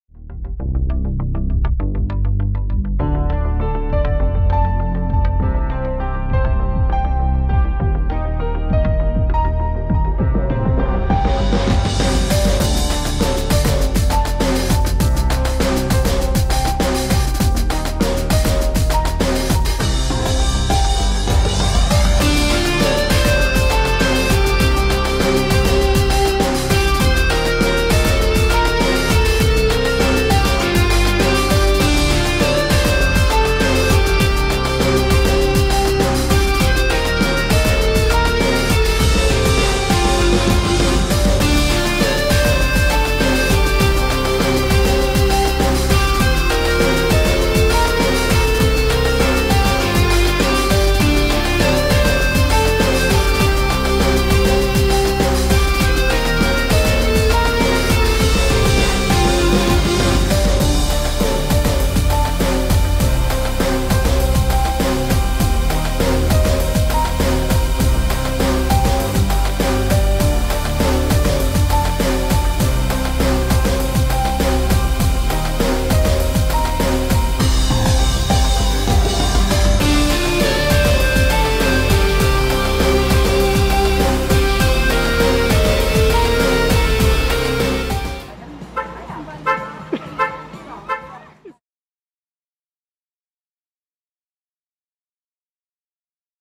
Imagevideo: Model des Monats - Bernadette Kaspar
Ein Imagevideo der Bergischen Innovations GmbH
Bernadette Kaspar ist das Model des Monats des Werkstatt-Produkte.de Kalenders 2013.